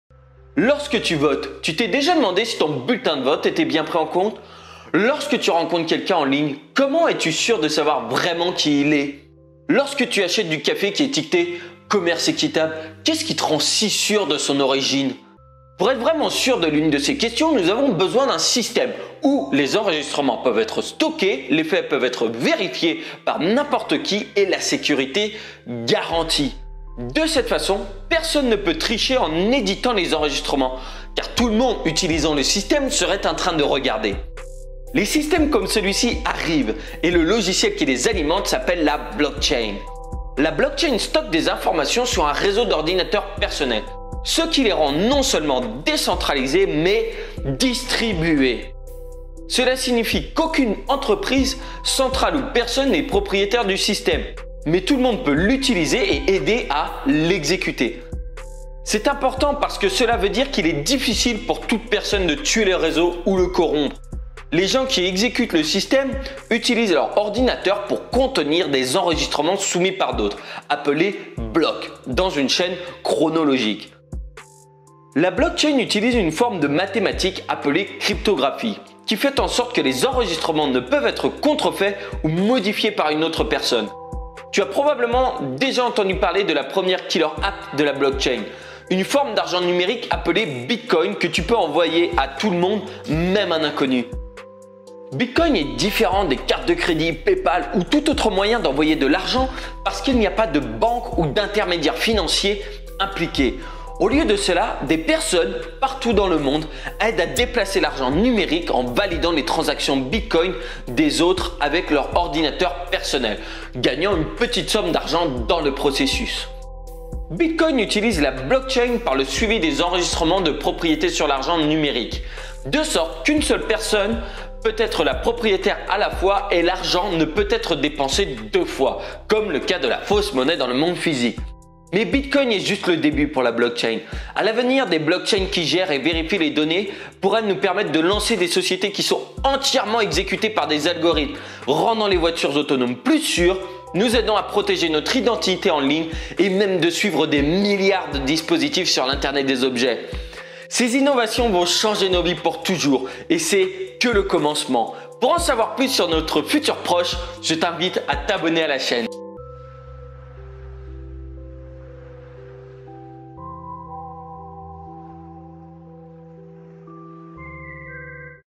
Tout Savoir sur La Blockchain, Le Bitcoin et Les Cryptos
Nous vous encourageons à partager cette vidéo a vos amis pour qu'ils en profitent aussi.

Ça devrait vous expliquer les bases de fonctionnement de la blockchain et du bitcoin.